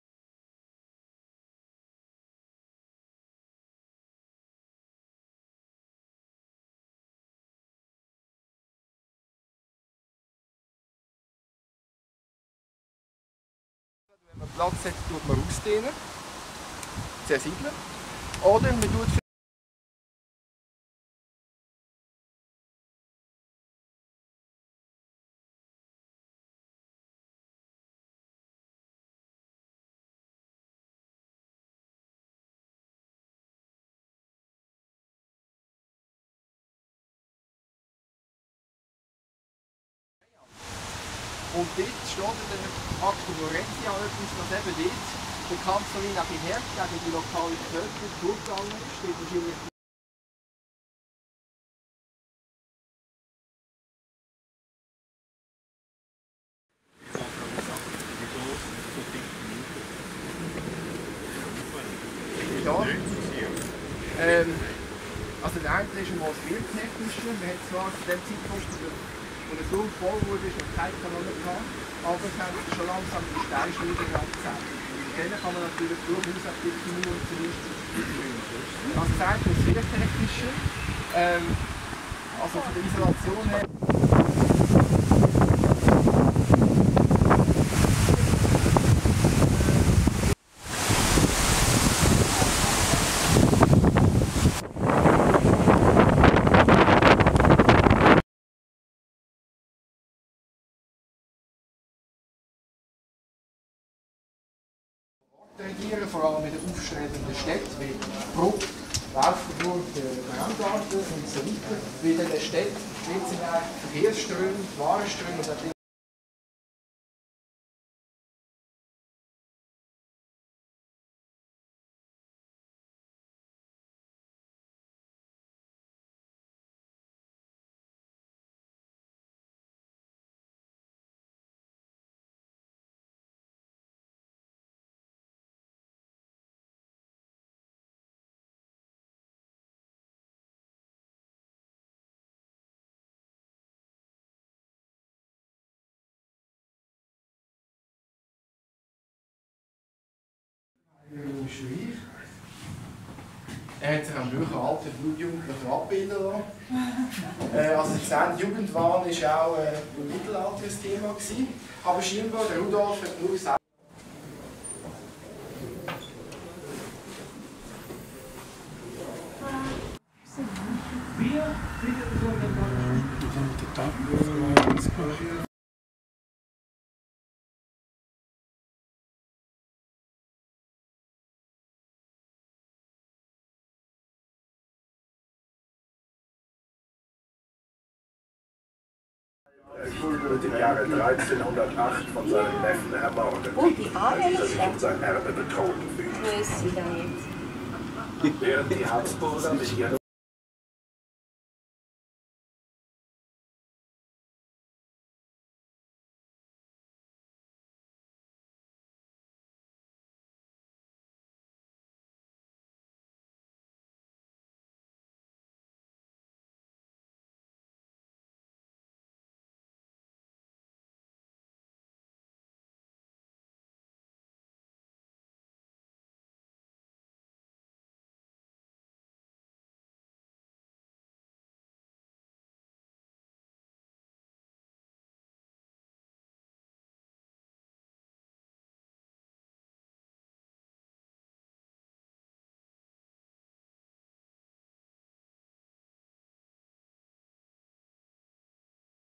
Die Habsburg
Bilder eines Ausflug zur Habsburg, des Stammhauses der Habsburger-Familie.